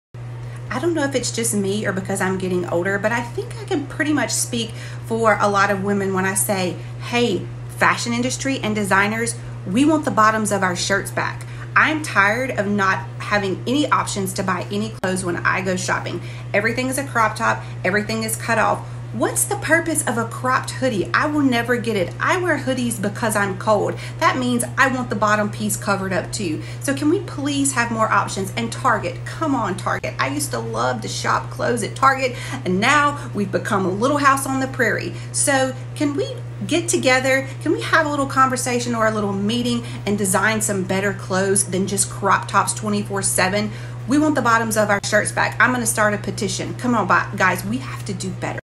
Fashion Industry.... Do Better!!!!
I think I speak for a lot of women when I say we want the bottoms of our shirts back! Fashion industry you have to do better! 99% of us women don’t want to wear crop tops we need more options!
fashion industry
do better